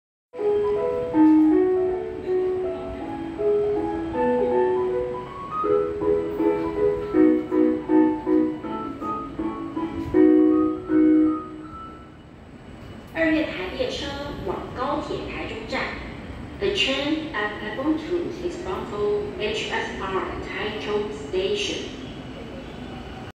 |安哥生活日誌| [台中捷運] 新進站音樂 往119高鐵台中站方向 (攝於111水安宮站)
拍攝路線: 台中捷運 綠線
台中捷運新進站音樂 往高鐵台中站方向

English Description
Taichung Metro New Departure Melody
Record Date: 6/13/2023
Record Line: Green Line
Vehicle Condition: Pending status
Camera Model: SONY HDR-CX405
我的DC: : 安哥生活日誌(陳奕安)#0792